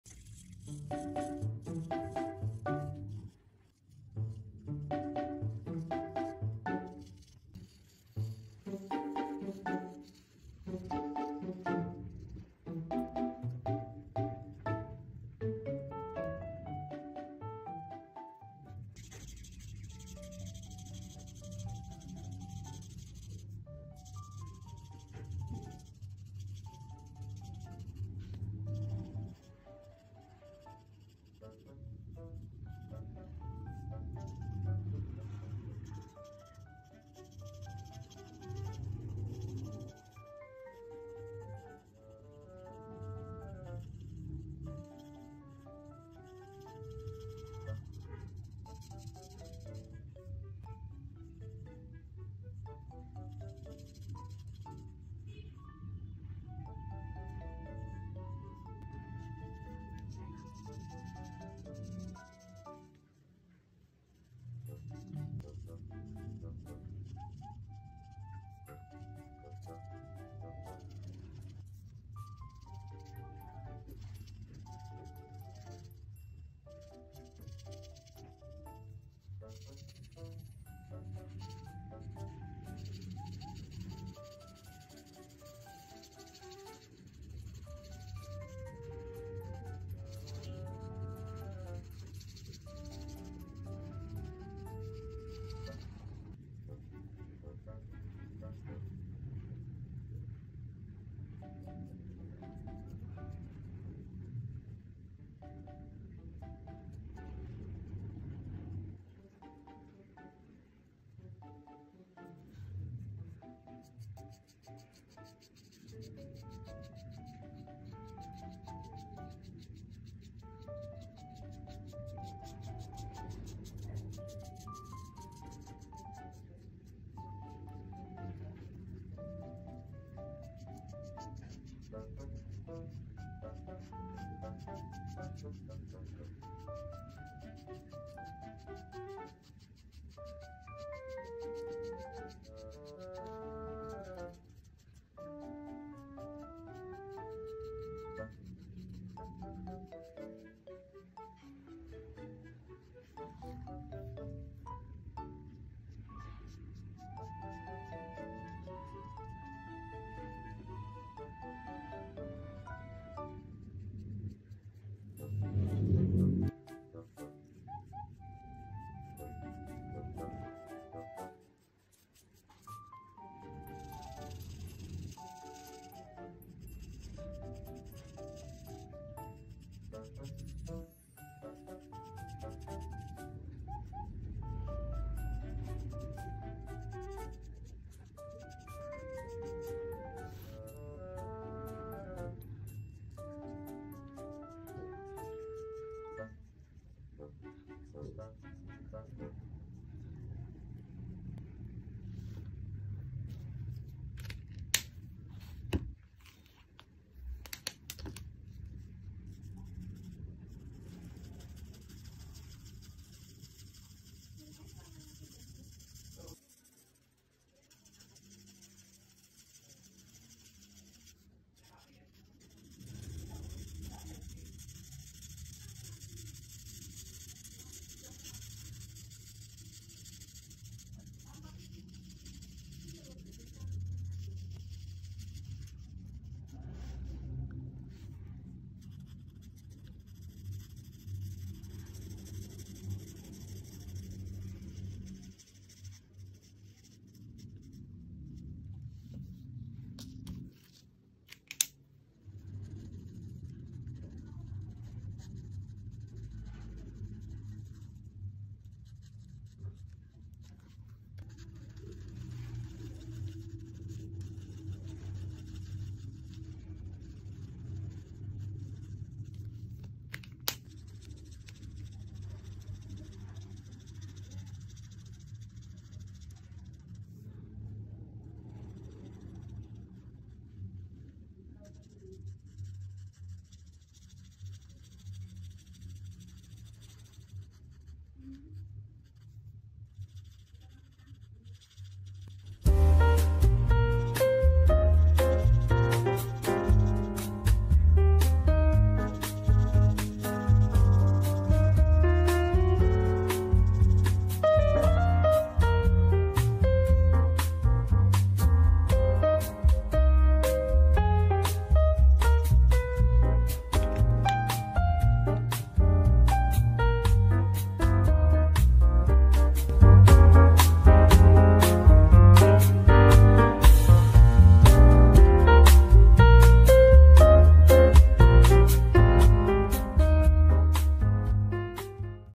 Coloring Trolls Cooper | Drawing Trolls | Drawing Tutorial
Hello Guys today i coloring Trolls Cooper
this guy so funny and nice buddy. hope you like guys